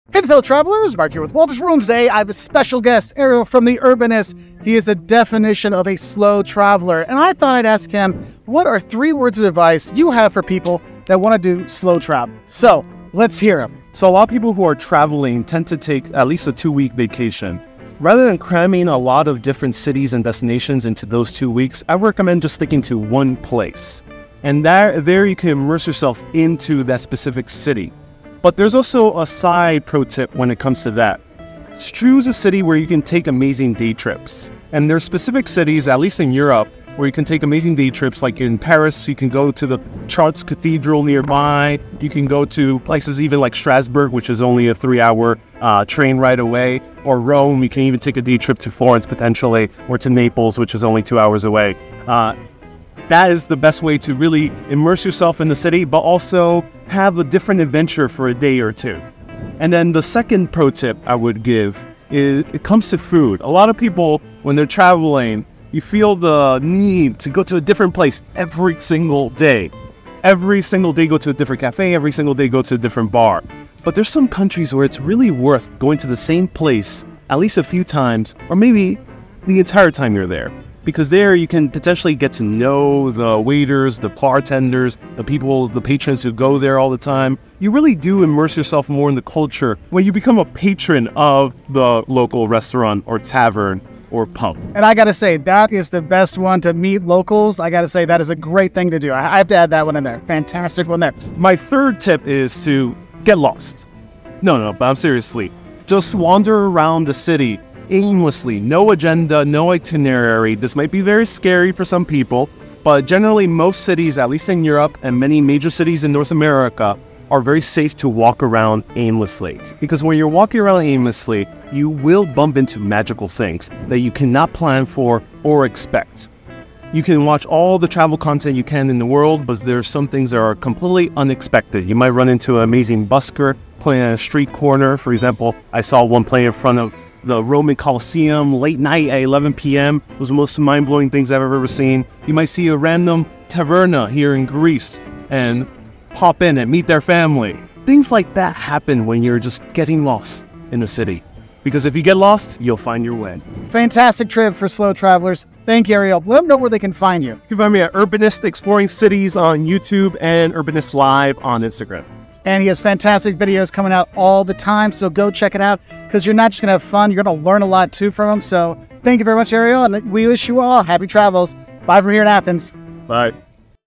What is "SLOW" Travel? with The Urbanist
Find Ariel at @UrbanistExploringCities on YouTube!
Are you thinking about slowing things down when you travel and really take time to get to know the culture of the city or country you are visiting? Well our friend Ariel Viera from The Urbanist has three suggestions to help you get the most ouf our your slow travel experience.
1. Find a good base city, 2. eat at the same place to make friends, 3. Get lost in the city to find out more abou tthe city.
Filmed in Athens, Greece